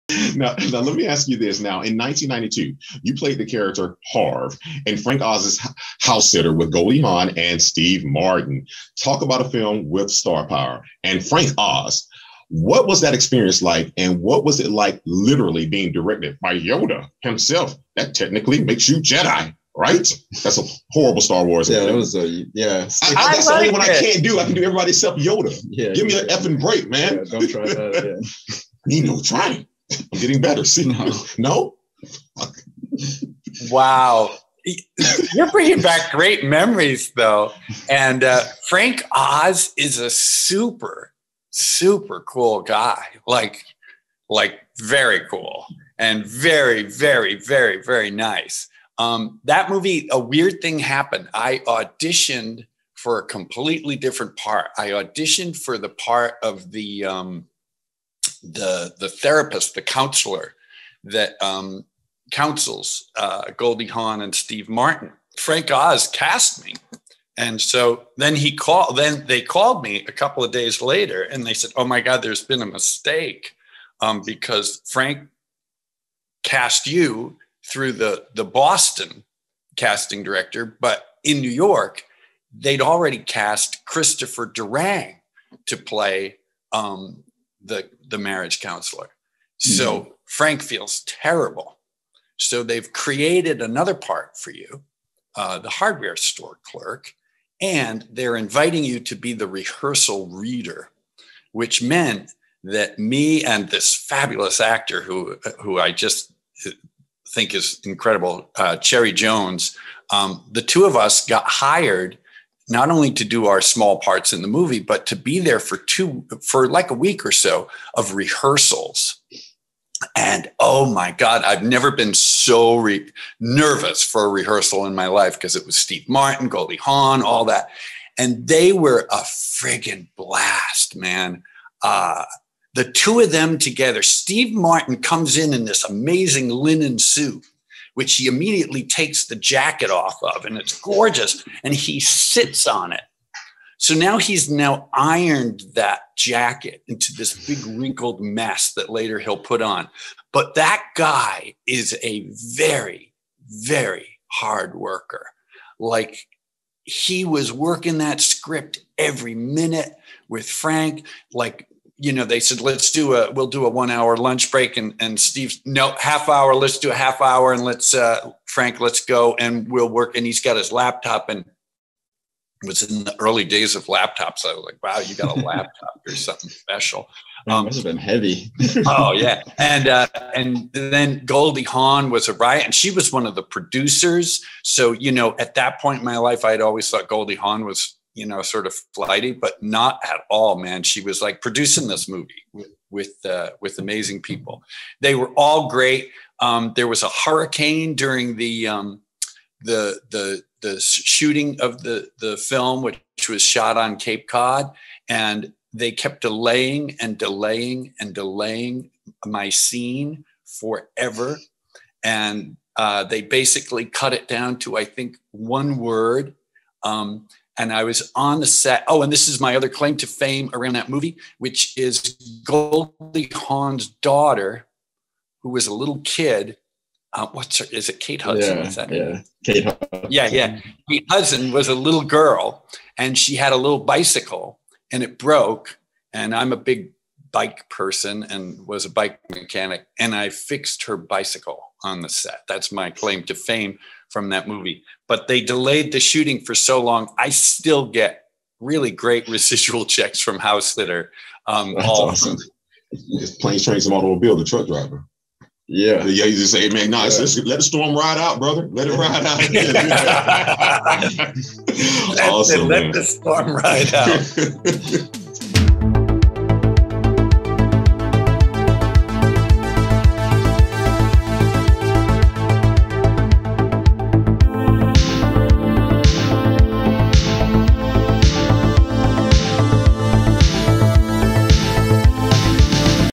Ken Cheeseman Talks About Working On Housesitter
Ken Cheeseman remembers working on Housesitter and how impressed he was by Steve Martin's commitment.

New movie-related content weekly!